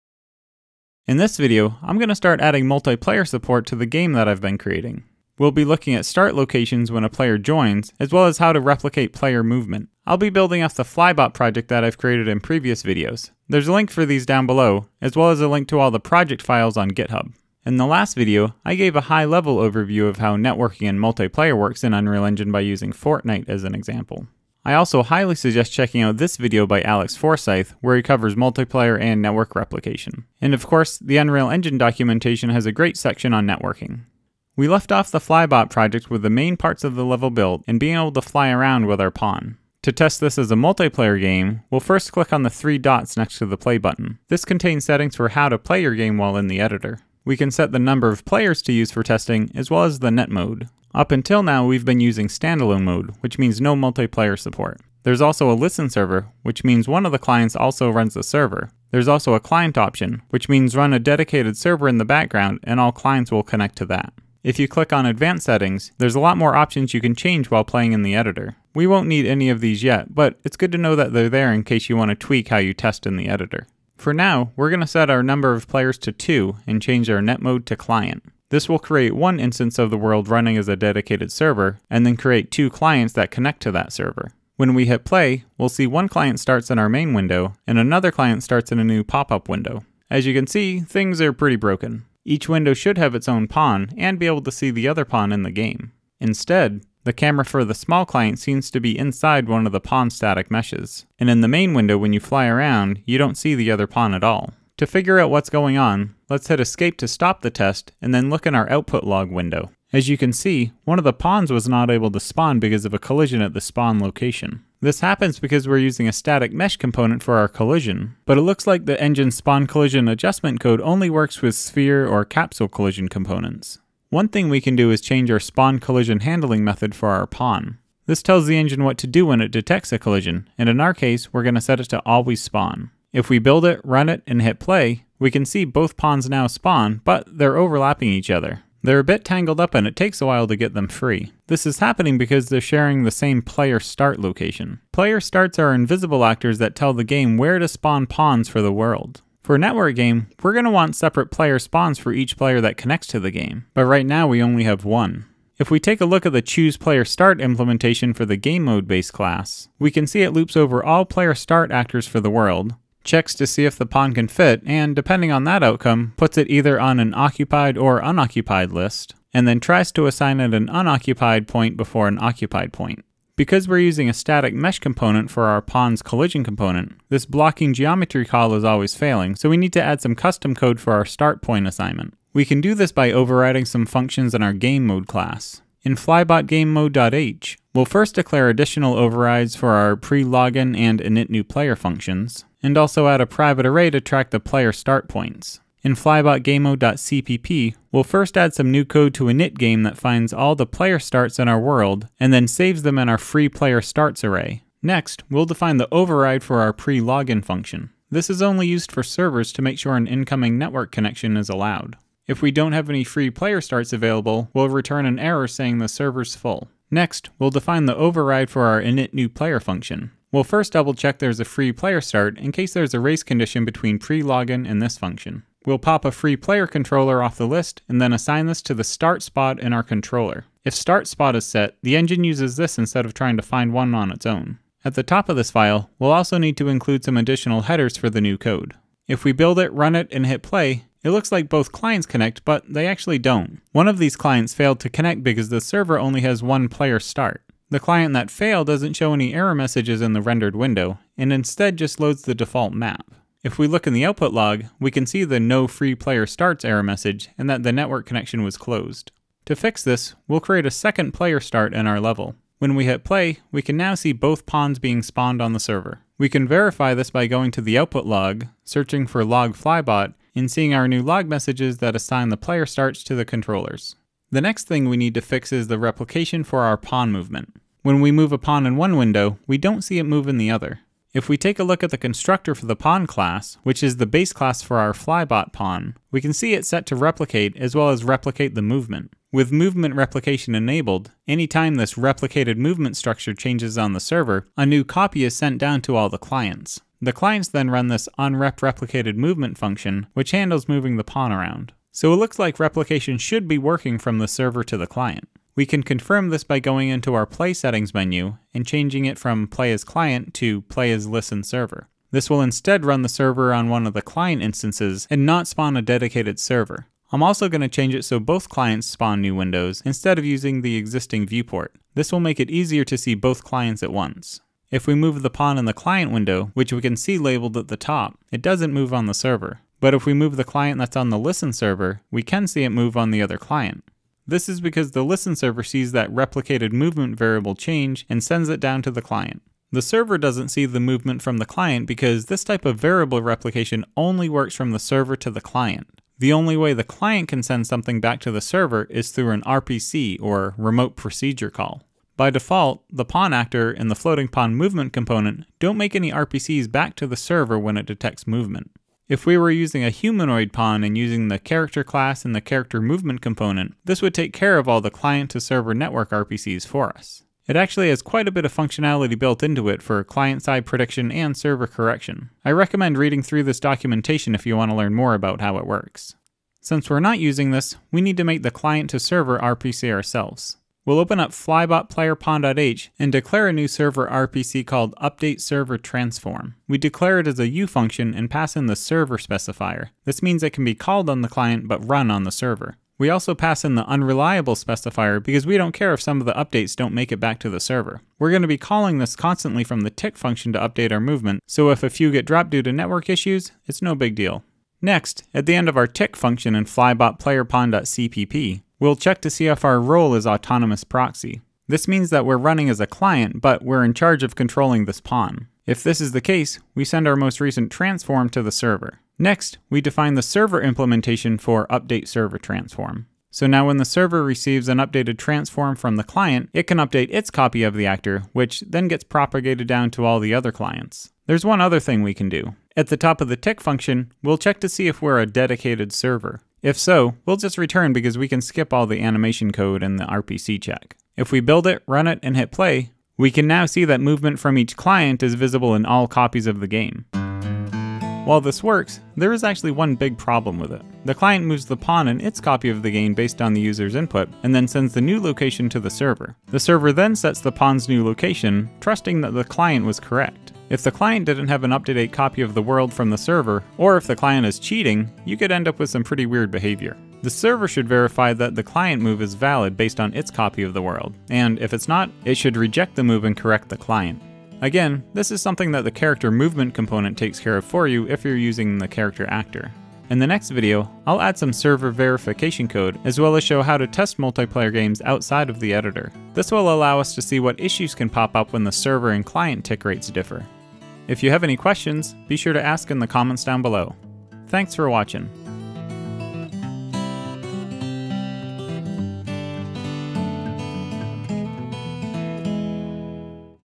Adding Multiplayer Support in Unreal Engine 5
In episode 15 of the UE5 game development series, we'll start adding multiplayer support. We'll first look at how network players spawn and customize our PlayerStart assignments. We'll then learn how client and server movement replication works, and add a client to server RPC (Remote Procedure Call) so our movement is replicated.